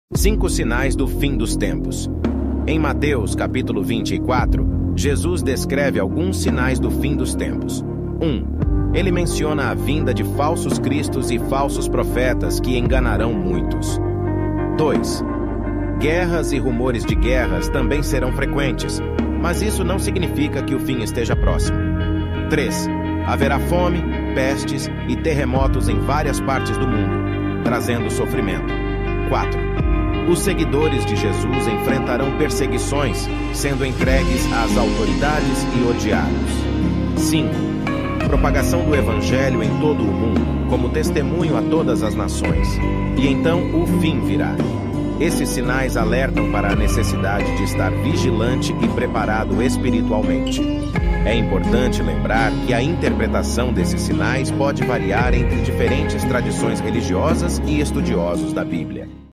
😱 5 Sinais do Fim dos Tempos!
Este vídeo revelador explora os 5 sinais proféticos do fim dos tempos descritos na Bíblia, trazendo uma compreensão mais profunda sobre o assunto. Através de análises minuciosas e embasadas nas escrituras sagradas, mergulhe em uma jornada fascinante sobre eventos que indicam a proximidade do fim dos tempos. Desde falsos profetas até desastres naturais, explore as revelações impressionantes que apontam para o cumprimento das profecias bíblicas. Prepare-se para ser surpreendido e desafiado em sua fé enquanto desvendamos os segredos contidos na Palavra de Deus.
😮 5 Sinais do Fim dos Tempos! jesus # shorts
Cinco sinais do fim dos tempos em Mateus Capítulo 24 Jesus descreve alguns sinais do fim dos tempos um ele menciona a vinda de falsos cristos e falsos profetas que enganaram muitos dois guerras e rumores de guerras também serão frequentes Mas isso não significa que o vinho esteja próximo 3 haverá fome pestes e terremotos em várias partes do mundo trazendo sofrimento quatro os seguidores de Jesus enfrentarão perseguições sendo entregues às autoridades e odiados sim propagação do evangelho em todo o mundo como testemunho a todas as nações e então o fim virá esses sinais alertam para a necessidade de estar vigilante e preparado espiritualmente é importante lembrar que a interpretação desses sinais pode variar entre diferentes tradições religiosas e estudiosos da Bíblia

👍Leia a BÍBLIA Diariamente!